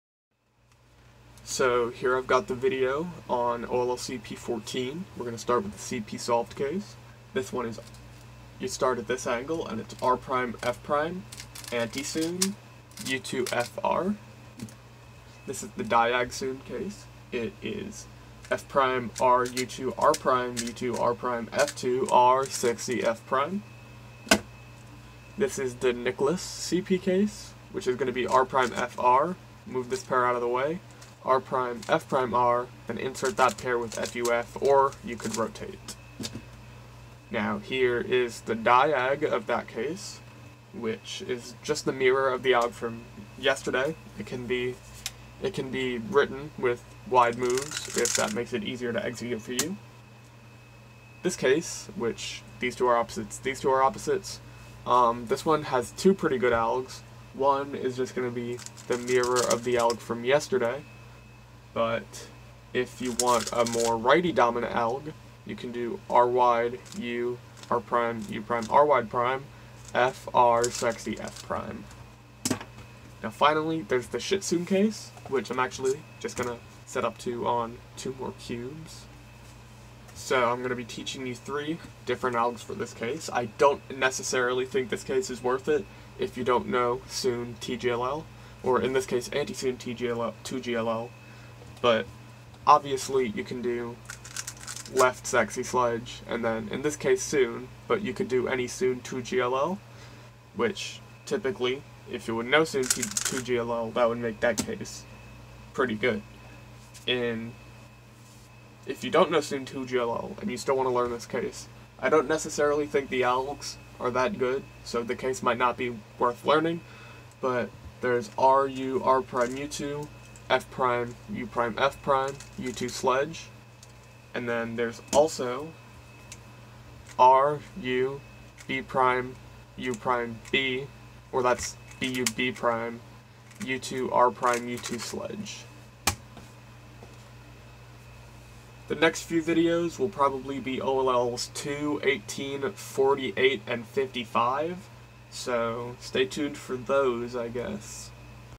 OLLCP 14
ollcp algs for oll 14
R' F' R U2 R' U' R U' R' U2 F R

F' R U2 R' U2 R' F2 R2 U R' U' F'

R' F R U R' F' R F U' F'

F' U' r' F r2 U r' U' r' F r

r U R' U' r' F R2 U R' U' F'
F' U' L' U2 L U L' U' L F

L' U' L U L F' L' F L' U' L U' L' U2 L
R U R' U2 F' U' F U2 R' F R F'
R U B U' B' U2 R' U2 R' F R F'